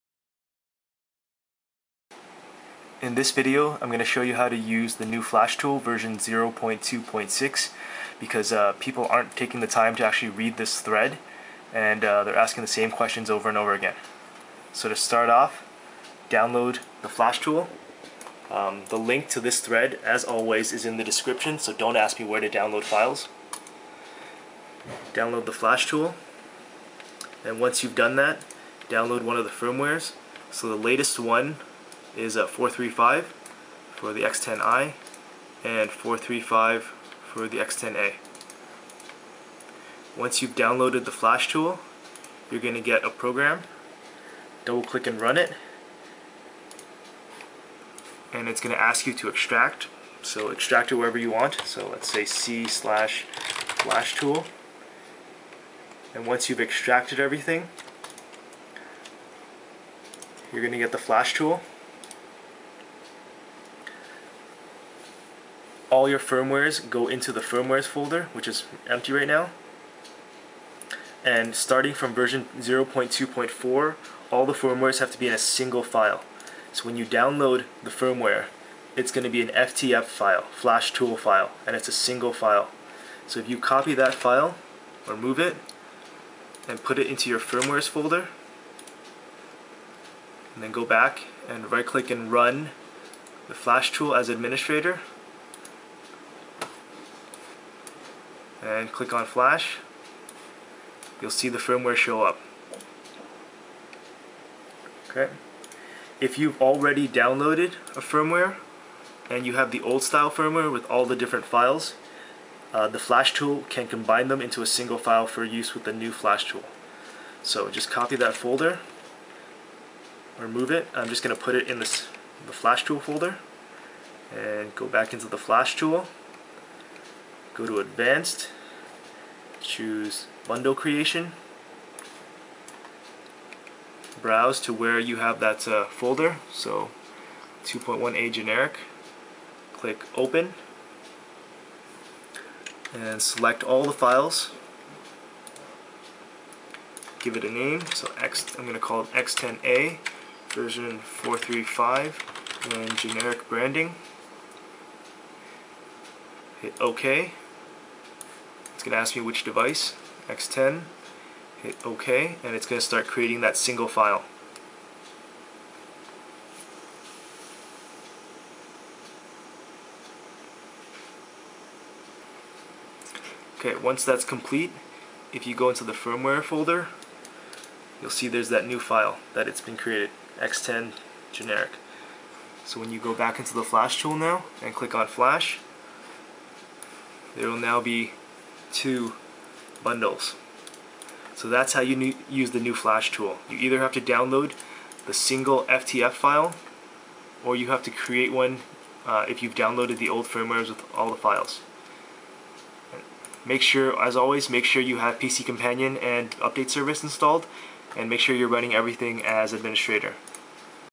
Using Bin4ry's Graphical Firmware Flashing Tool v0.2.6 for the Xperia X10
A short walk-through of Bin4ry's graphical flash tool v0.2.6.

It now uses SINGLE firmware files (.FTF) instead of a folder full of files.
If you've downloaded the older firmwares, the graphical tool can create FTF files by combining them.

You need the Flash Tool AND a firmware.

The latest firmware, as of 2011 February is build 2.1.A.0.435.